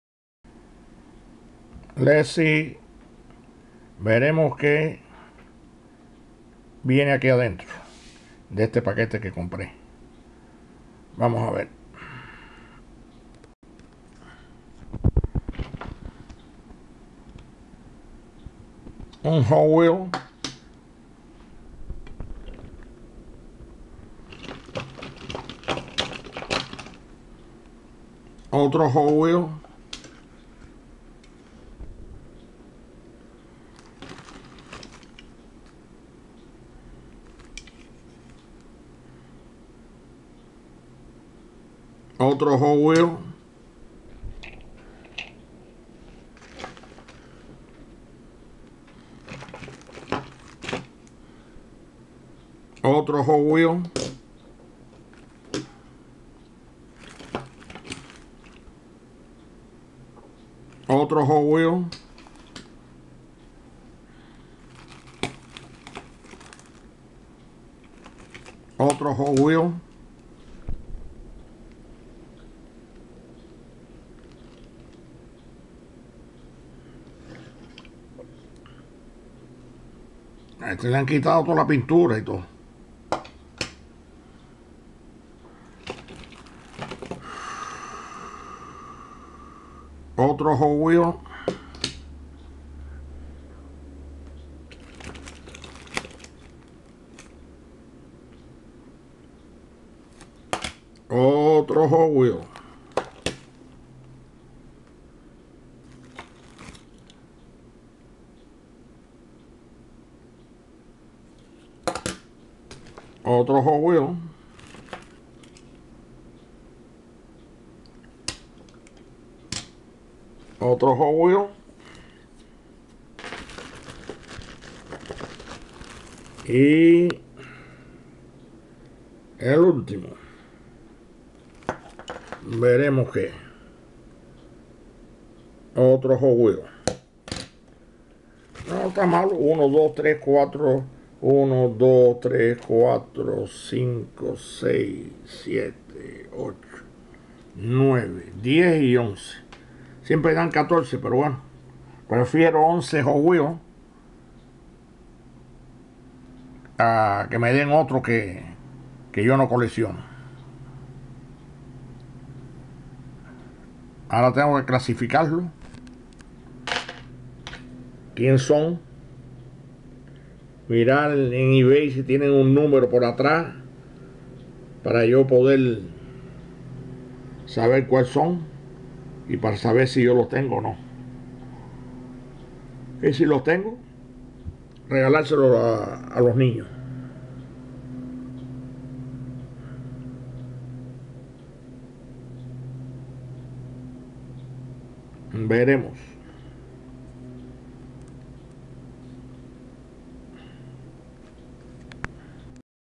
HOT WHEELS PACKAGE
I BOUGHT THIS PACKAGE AND BROUGHT 11 HOT WHEELS